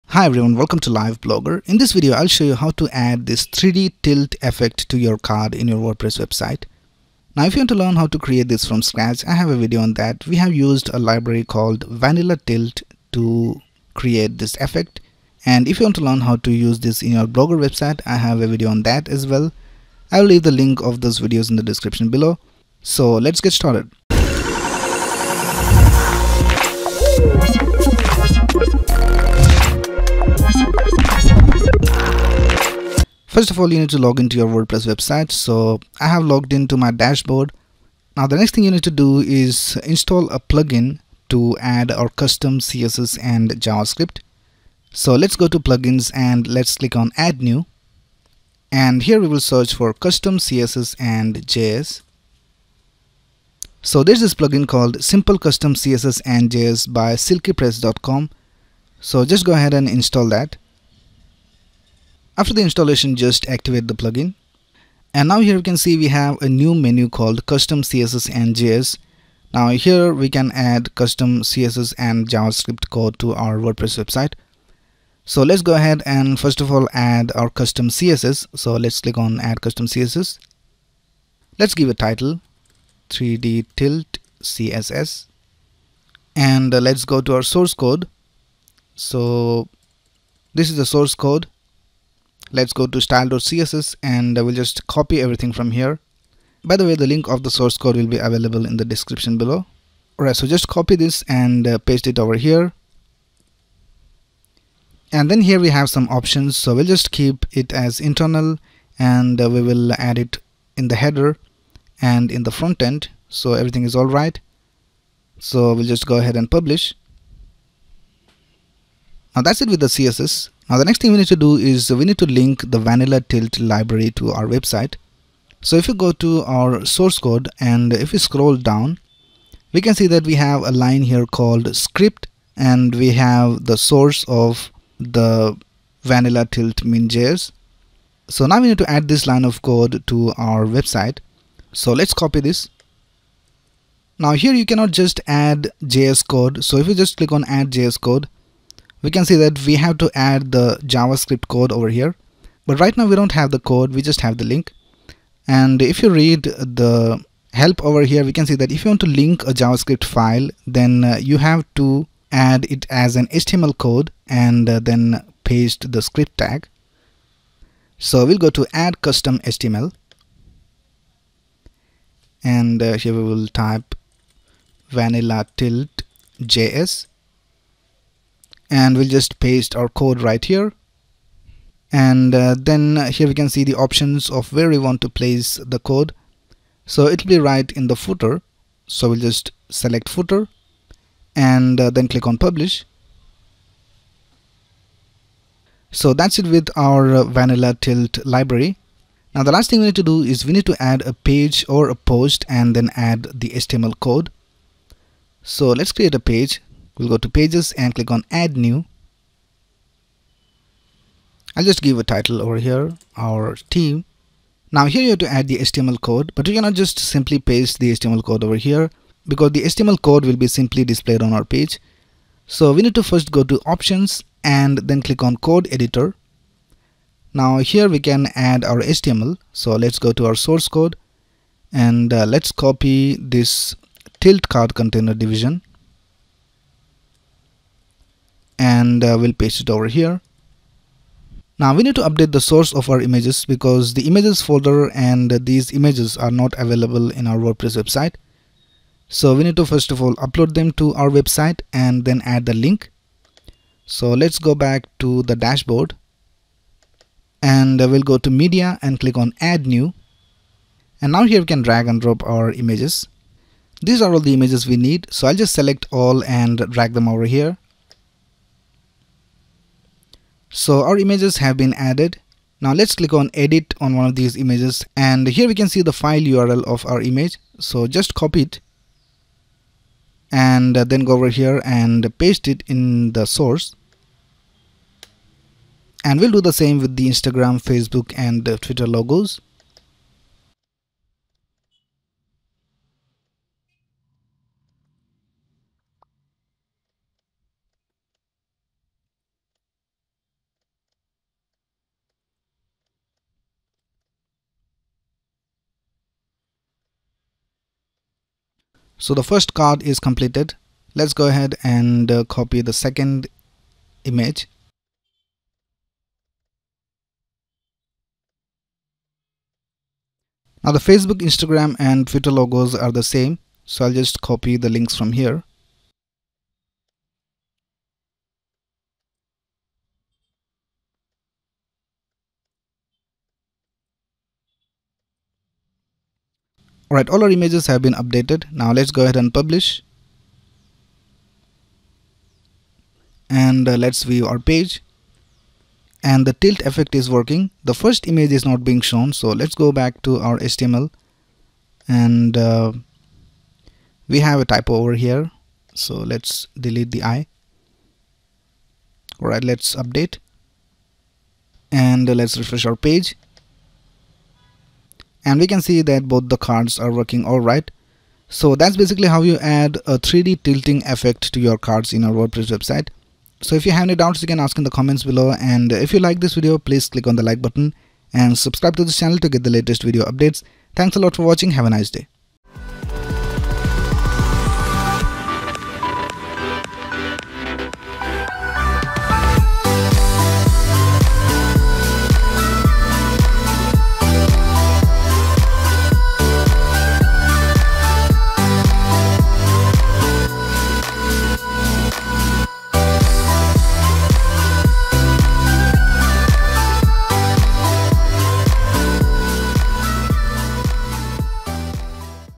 How To Add Awesome 3D Tilt Effect To A Card In WordPress Website
Hi, in this video, I will show you how to add a 3D Tilt Effect to a card in your WordPress website.